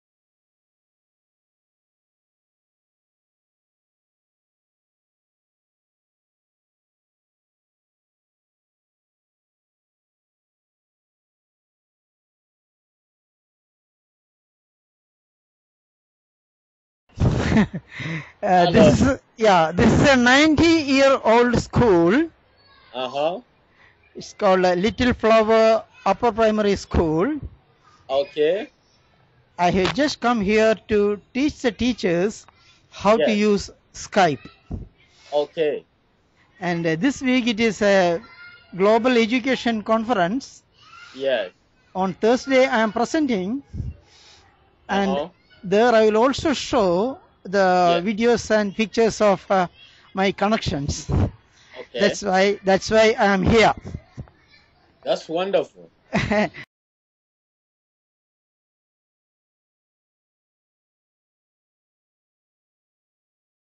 Nalla Padam at Little Flower U.P. School, N. Kaloor, Ernakulam
On request of Mrs. Anne Vackachan, Head Mistress of LF UPS, I invited Jairus Makambi of Kenya to connect with L.F. U.P. School. Now, Kenyan children of will connect with LFUPS students and learn together using Skype for good.